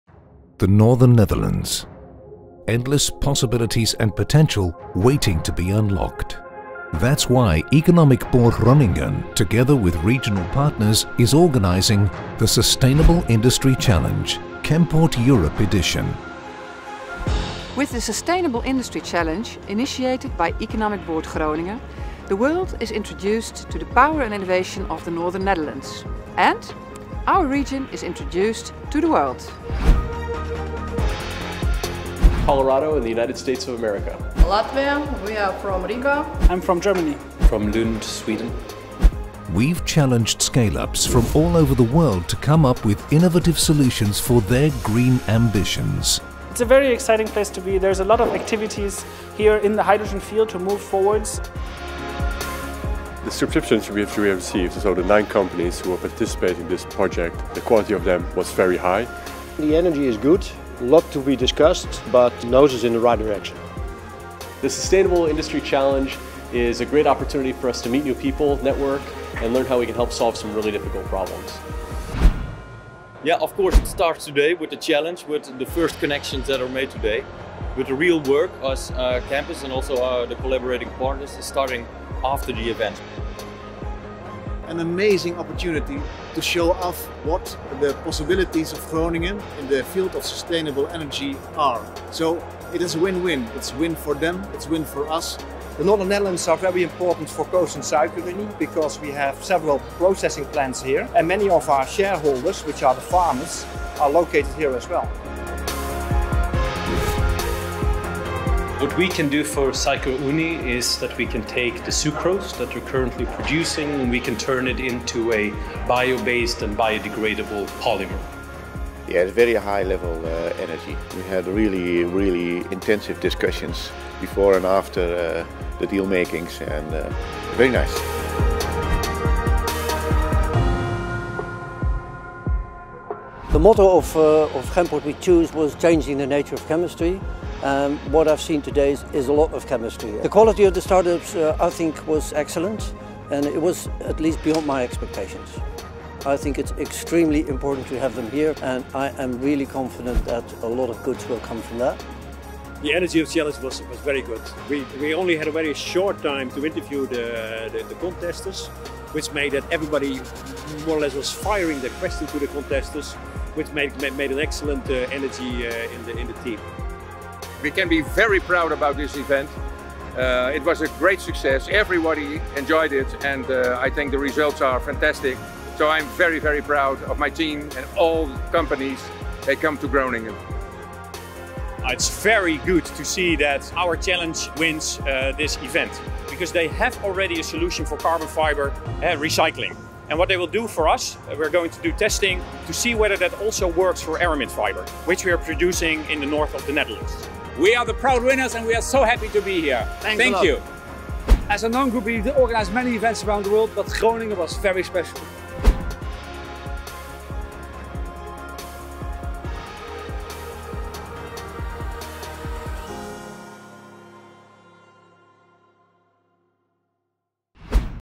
Aftermovie Sustainable Industry Challenge 2020
In this film we look back on the two-day event of the Sustainable Industry Challenge in January 2020. Economic Board Groningen is the initiator of the Sustainable Industry Challenge, Chemport Europe edition, from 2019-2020. The partners are the Campus Groningen, NOM, Groningen Seaports, and the provincial executives of Groningen and Drenthe. All the organisations are part of Chemport Europe, the ecosystem for a green chemical sector in the northern Netherlands. This major competition used challenges formulated by industrial companies to call on innovative companies to come up with specific innovations in the area of making recycling, hydrogen or green chemistry more sustainable.